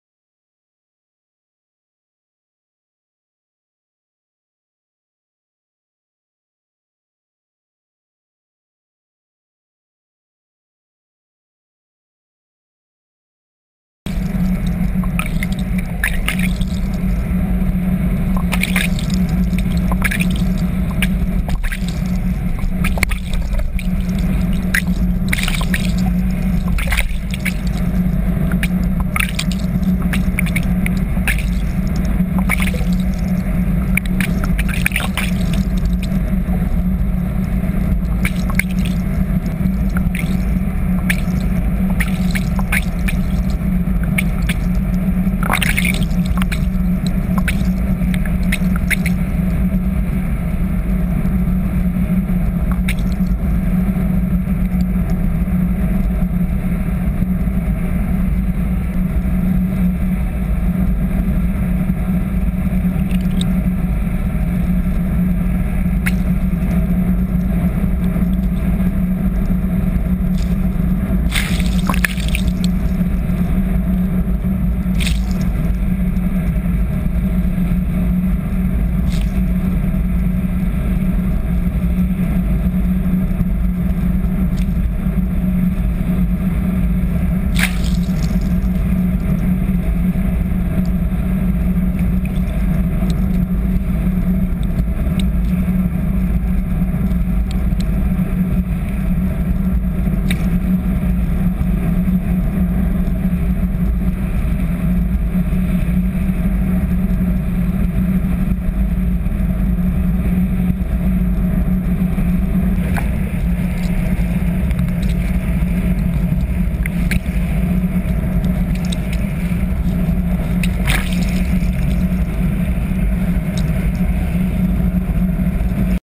Big Mouth Movie
Tormenter's Hottest Marlin Lure that raised the 800lb winning fish at the BIG ROCK Tournament in 2007. Check out the action on this bait.